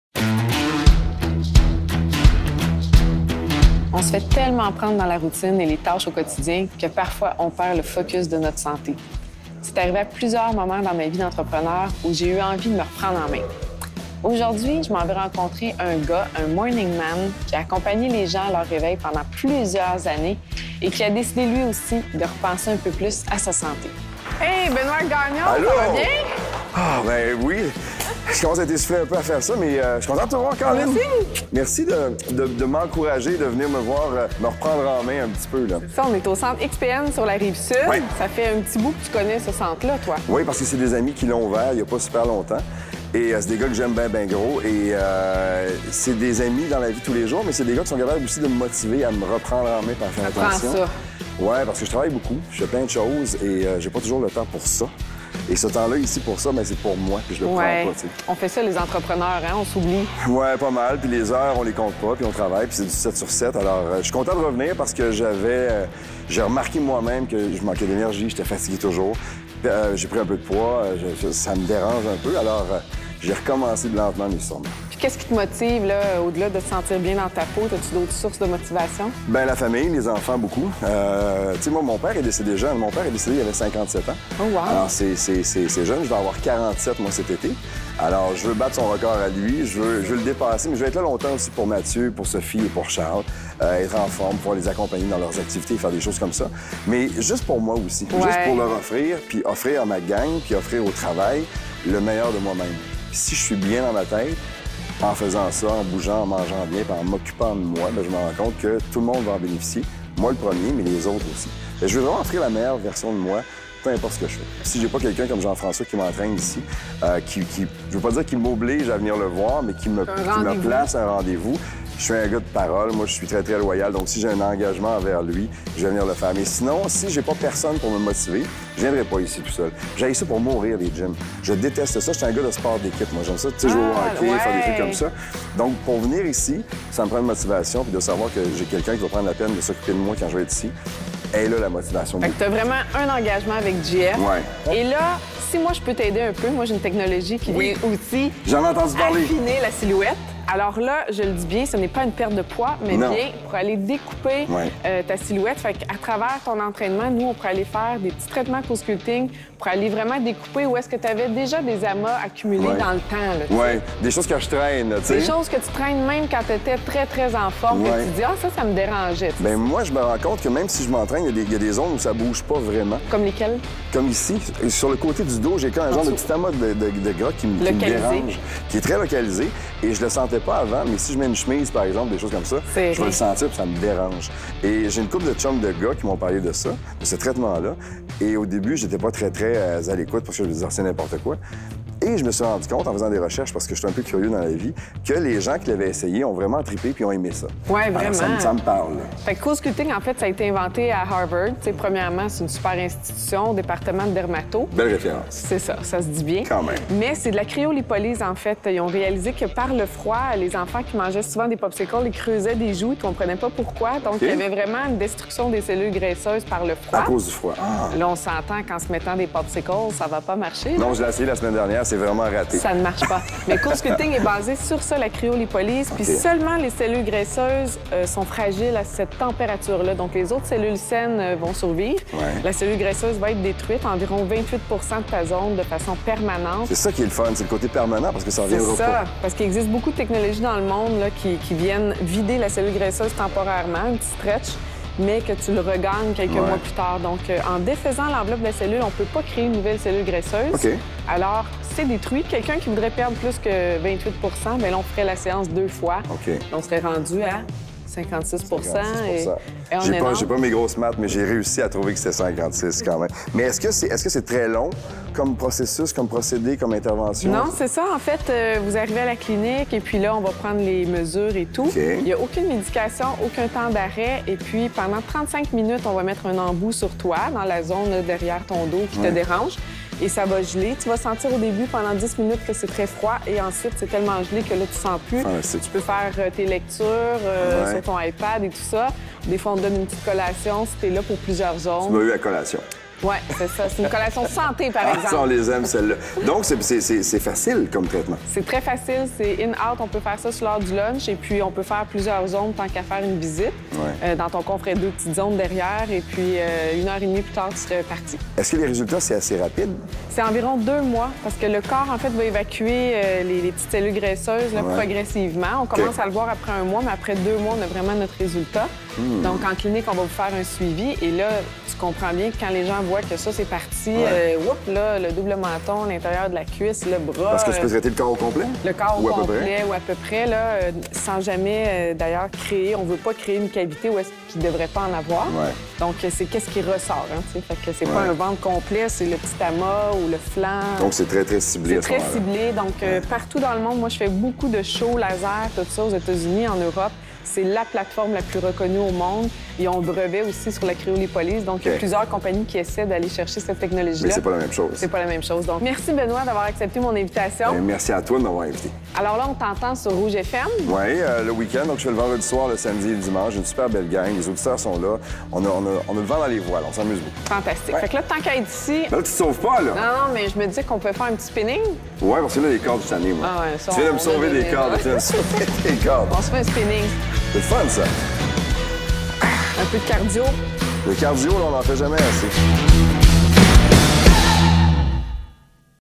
Benoît Gagnon - Gym de la peau #53
Marilyne surprend BENOÎT GAGNON en salle de gym!
L'animateur, qui désire perdre quelques petits kilos localisés et affiner sa silhouette, s'informe au sujet de la technologie CoolSculpting... et en fait l'essai sous vos yeux!